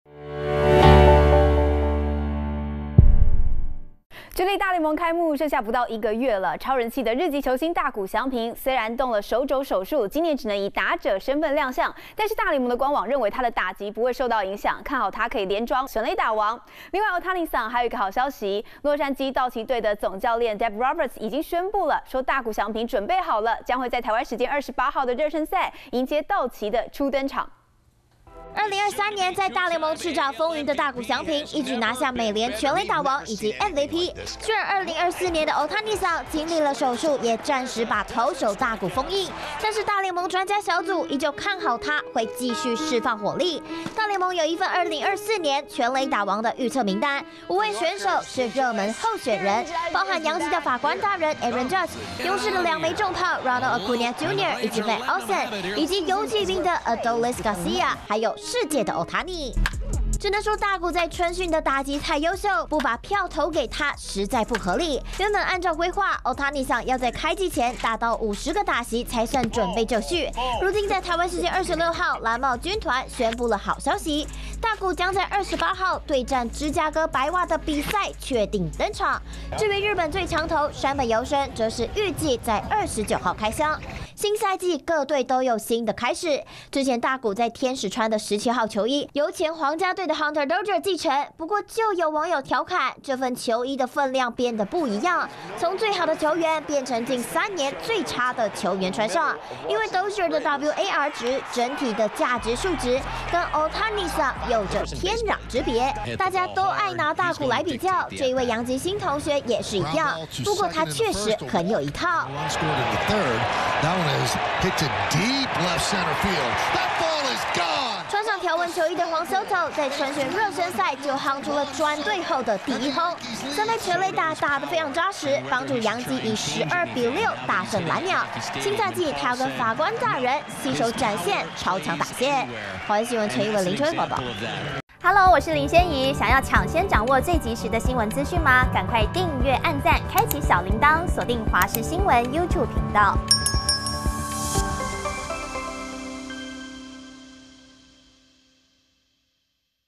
大谷「單刀出鞘」沒問題！ MLB看好連莊全壘打王｜華視新聞 20240226
距離大聯盟開幕剩下不到一個月！超人氣的日籍球星大谷翔平，雖然動手肘手術，今年只能以「打者」身分亮相，但是大聯盟官網認為他的打擊不受影響，看好他可以「連莊」美聯全壘打王！另外還有個Ohtani桑的好消息，洛杉磯道奇的總教練羅伯茲宣布，大谷翔平已經準備好了，將會在台灣時間28日的熱身賽，迎接道奇的初登場！